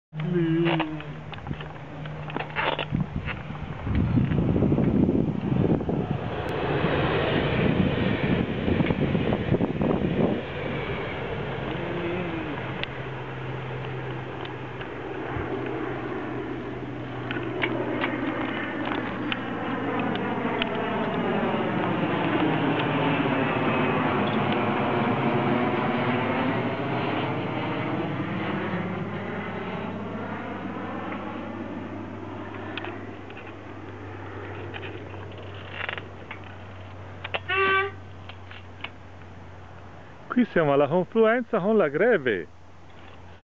CONDURRE PER CONDURSI: CICLOTERAPIA: BICI+AEREO+TRENO+FIUMI
Alexander Vismara per la prima volta da solo in bicicletta vede un treno venire controcorrente, poi un aereo incrociare in cielo e infine si ferma alla confluenza tra i fiumi Arno e Greve.